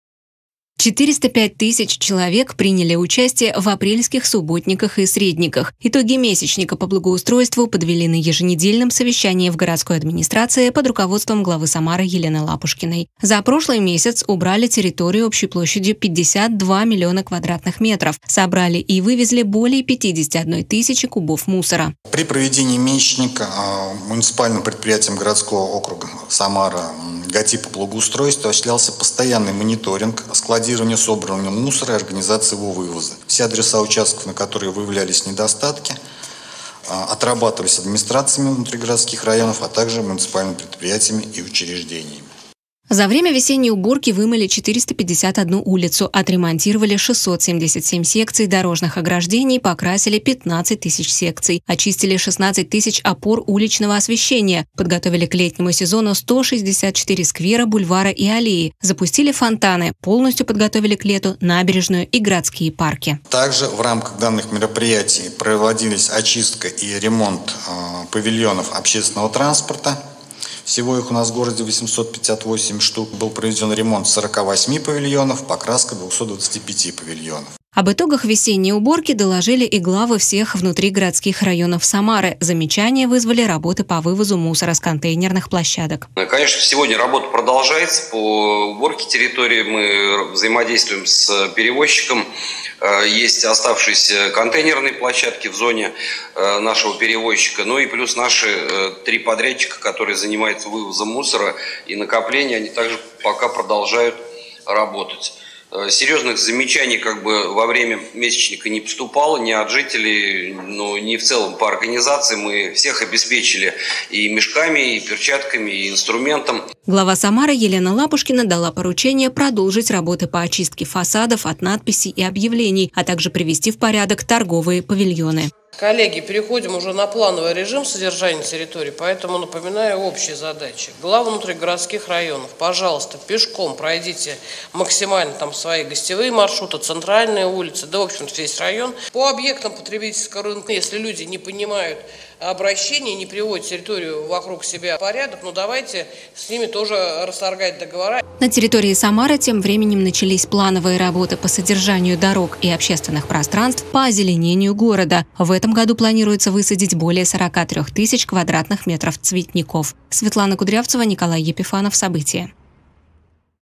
В Самаре подвели итоги месячника по благоустройству
Привели в порядок территорию общей площадью более 52 млн кв. метров. В городской администрации подвели итоги апрельского месячника по благоустройству. Сколько человек приняли участие в уборке, сколько вывезли мусора, и какие поручения главам районов дала мэр Самары Елена Лапушкина, расскажет Светлана Кудрявцева.